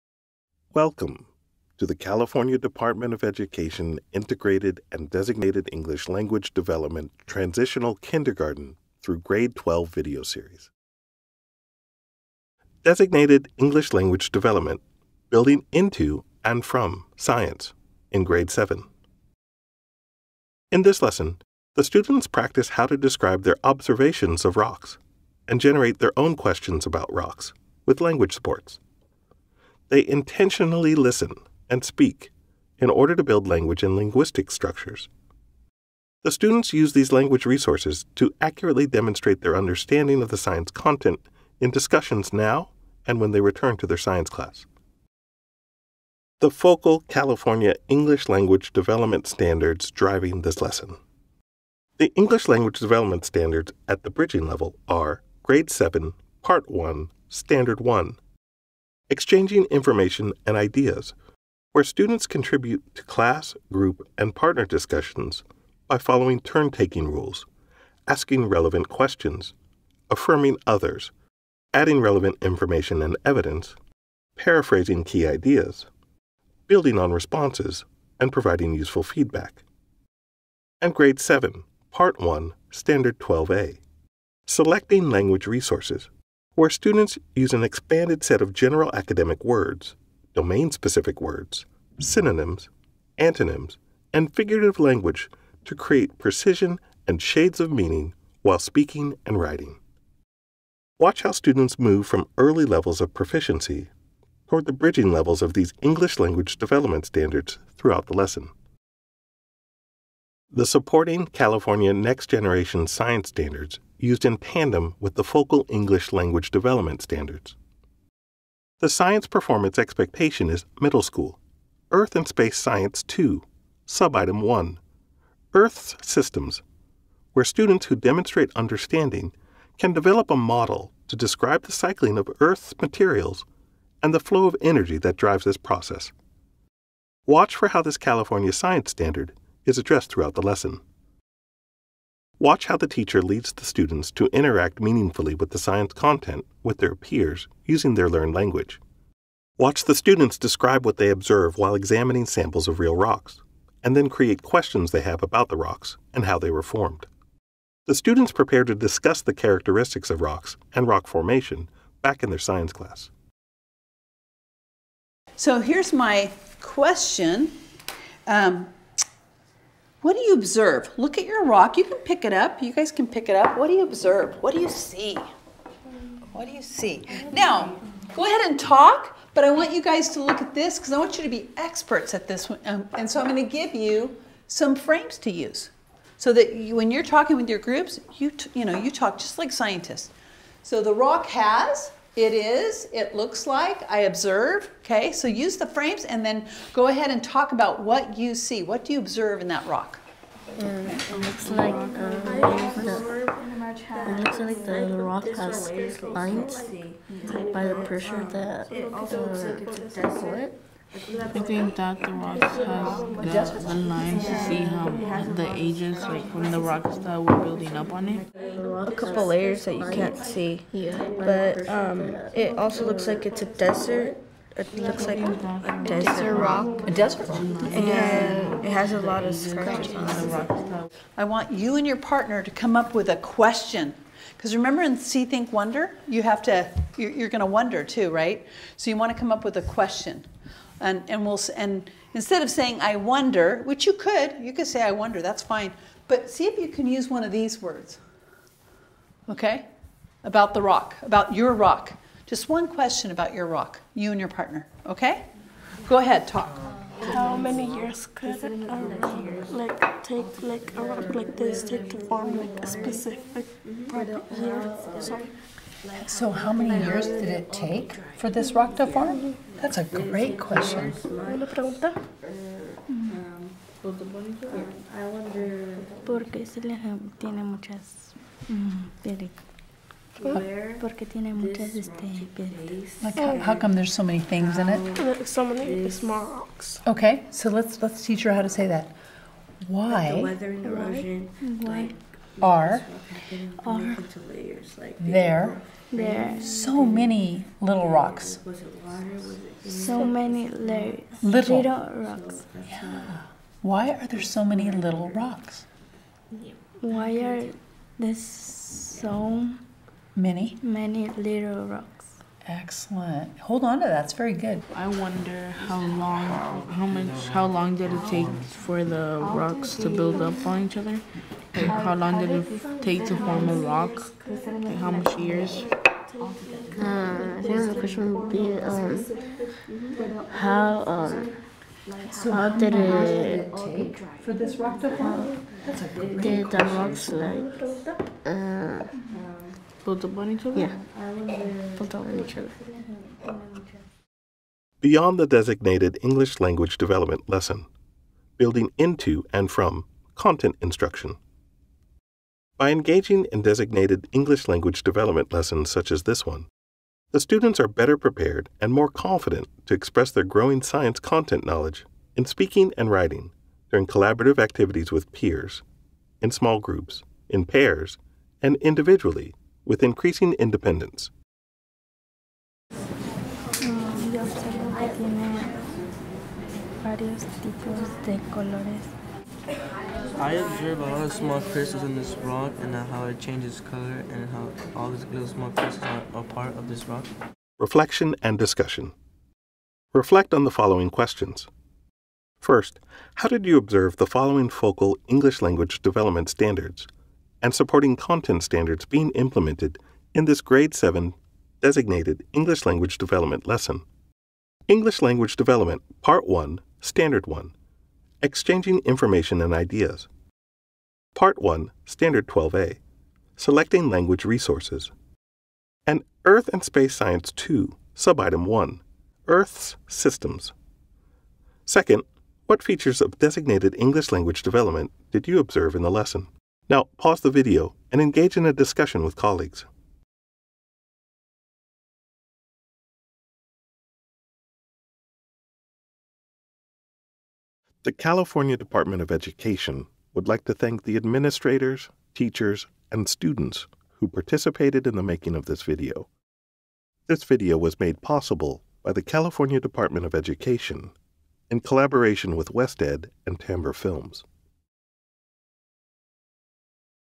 Grade 7 Science Designated ELD: Describing and Questioning
Designed with teachers in mind, this video shows a grade seven teacher leading English learner students in describing their observations of rocks and developing questions about the formation of the rocks, during a designated English language development lesson with science.